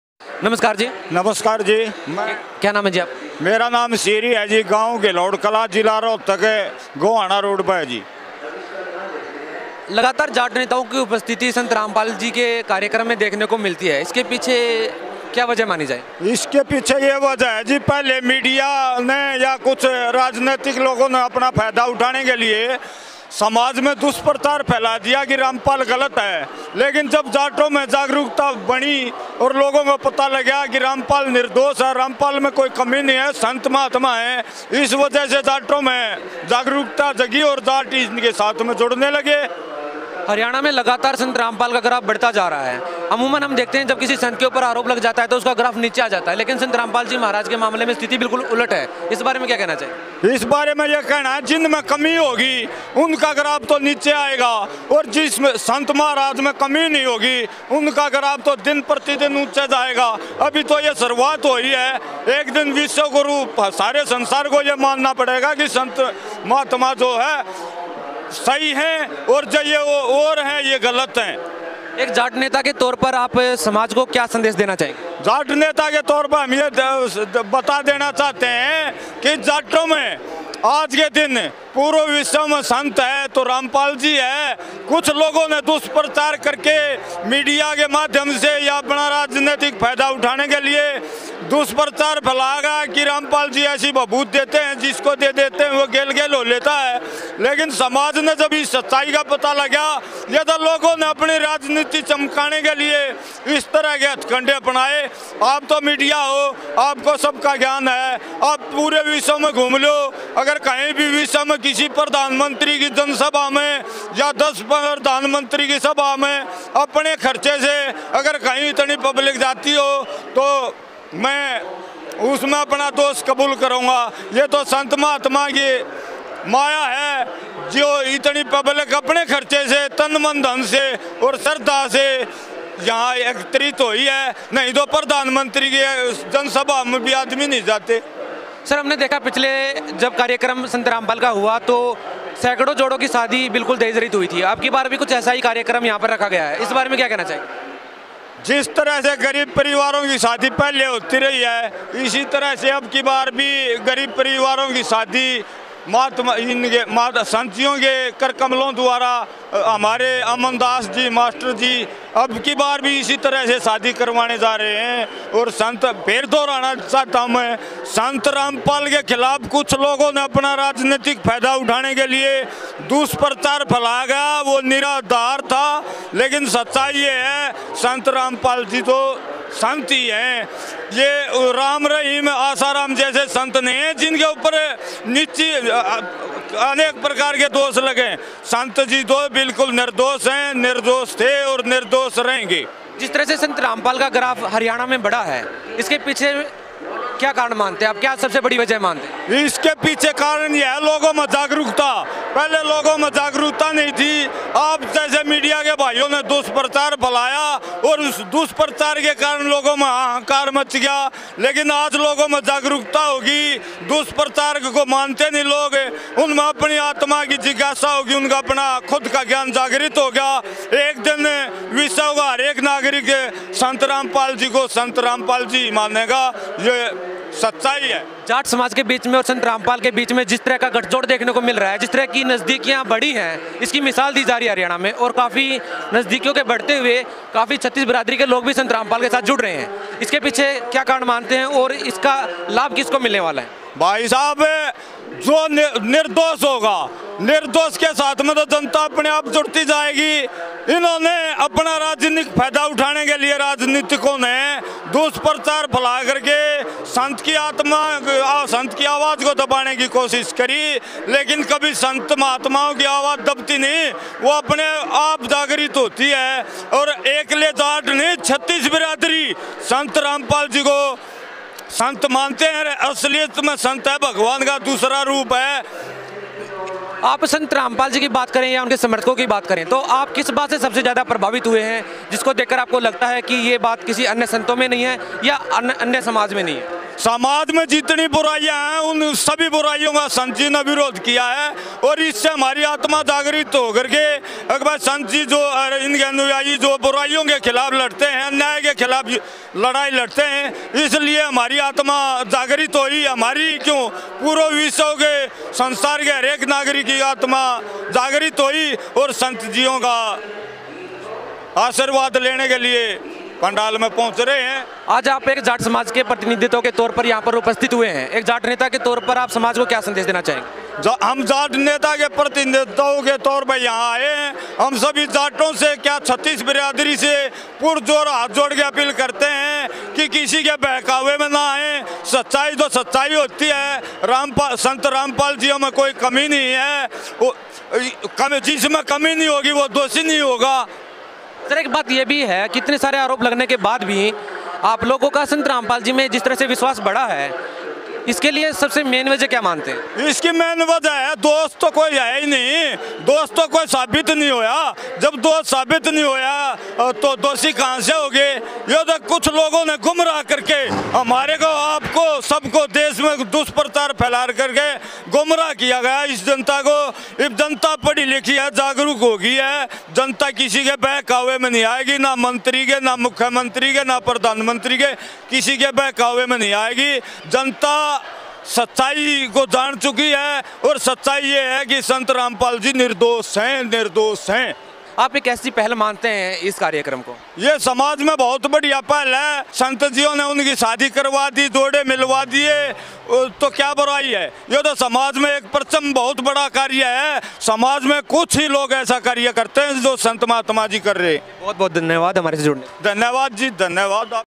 संत रामपाल जी की बढ़ती लोकप्रियता | Jaat Neta (सिरी सिंह - रोहतक) Interview|Kabir Parkat Diwas 2019.
संत रामपाल जी की बढ़ती लोकप्रियता | Jaat Neta
(सिरी सिंह - रोहतक) Interview | Kabir Parkat Diwas 2019.